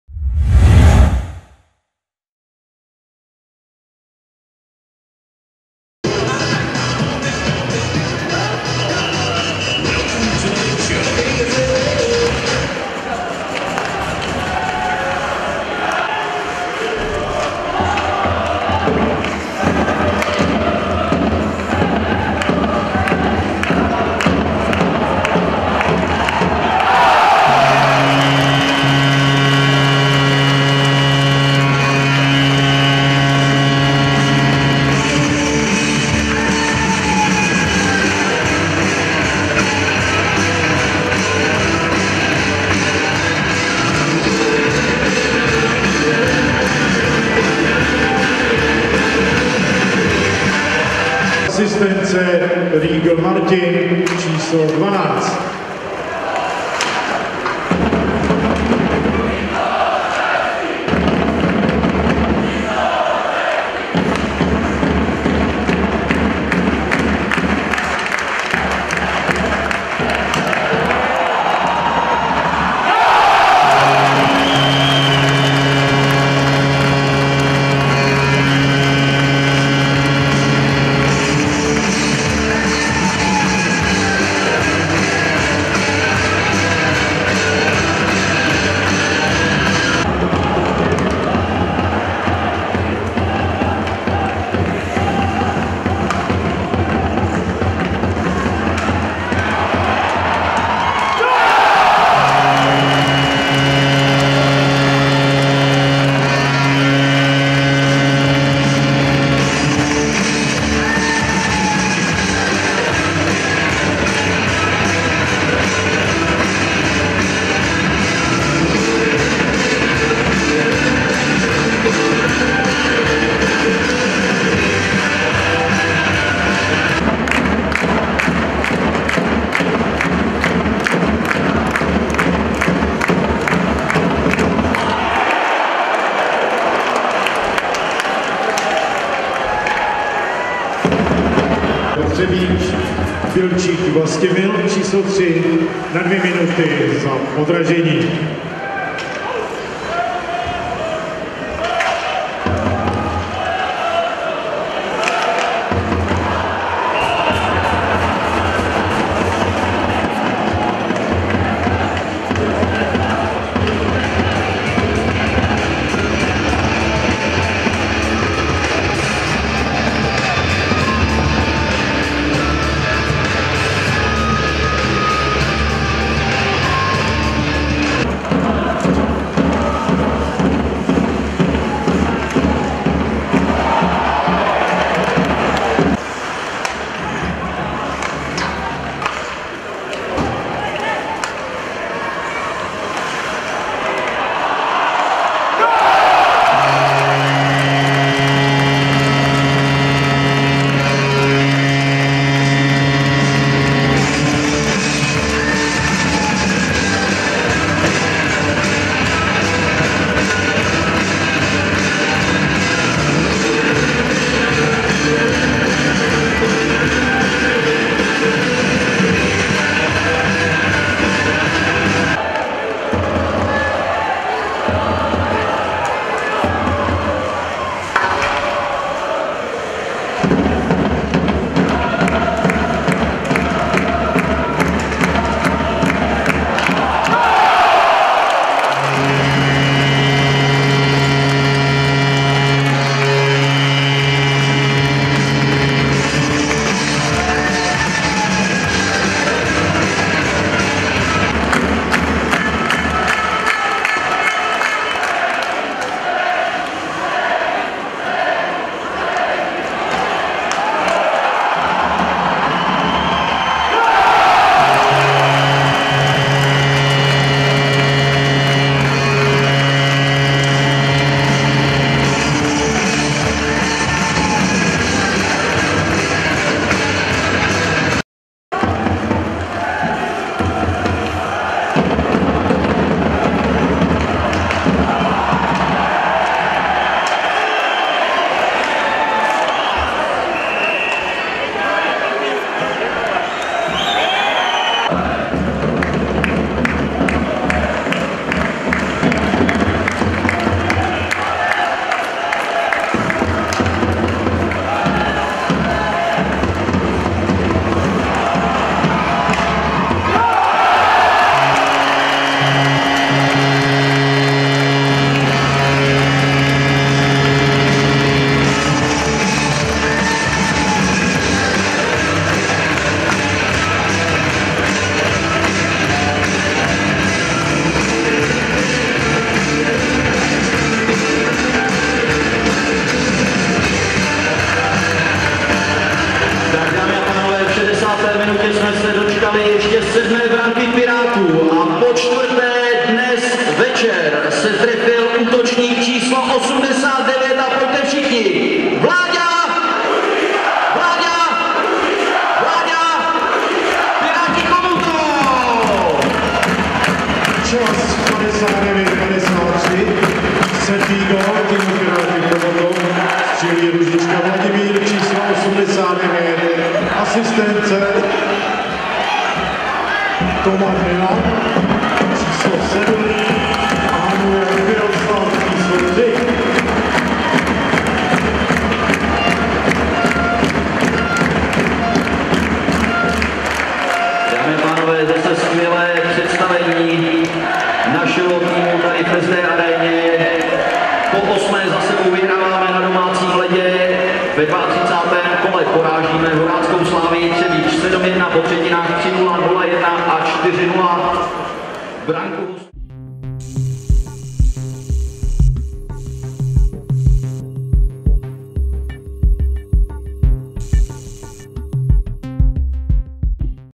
32. kolo: Piráti Chomutov - SK Horácká Slavia Třebíč 7:1 sestřih
Ve dvaatřicátém kole první hokejové ligy přivítali chomutovští Piráti na svém ledě Třebíč, kterou porazili 7:1. V utkání se čtyřikrát prosadil Vladimír Růžička, k němuž se dvakrát přidal Nikola Gajovský a jednou Marek Hovorka. Výborný duel odchytal Miroslav Hanuljak, který dostal pouze jeden gól.